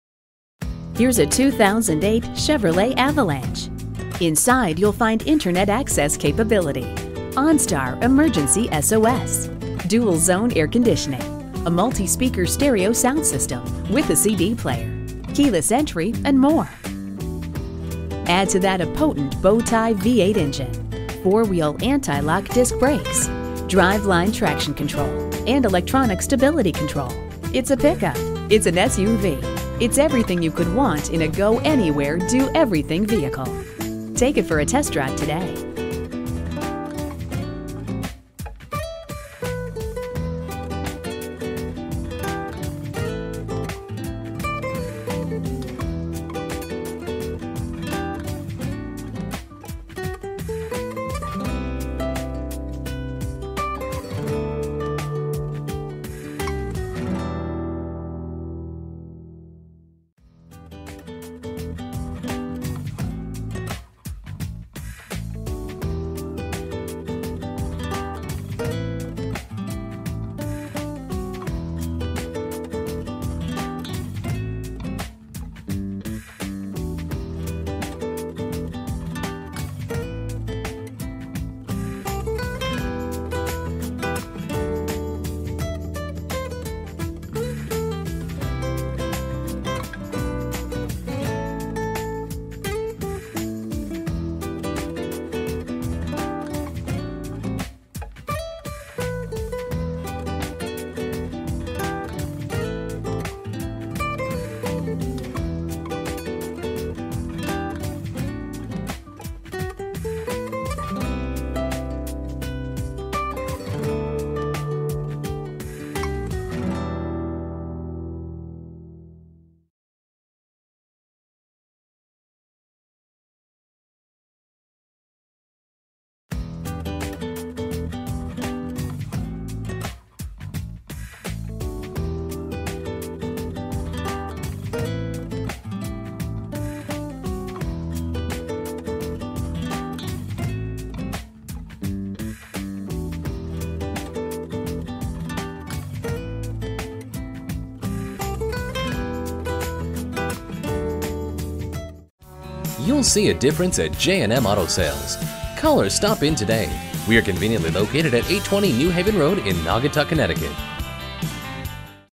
2008 Chevrolet Avalanche 4wd Naugatuck CT Hartford, CT #082576 - SOLD
Year: 2008
Make: Chevrolet
Model: Avalanche 4wd
Trim: 5d Crew Cab LT-1
Engine: -1 Cylinders
Color: Tan
Mileage: 89730
Stock #: 082576
VIN: 3GNFK12368G252576
An Impressive Condition, Four Wheel Drive Avalanche LT with an Accident-Free History Report and an Included Warranty! It's Fully Equipped with Tan Interior, Alloy Wheels, a Tonneau Cover, Privacy Glass, a Tow Hitch, Running Boards, a Bed Liner, Fog Lights, Power Windows, Power Door Locks, Power Mirrors, an Adjustable Steering Column, Cruise Control, a 5.3L V8 Engine, an Automatic Transmission and More! A Quality Truck at J&M!.

Address:
820 New Haven Rd.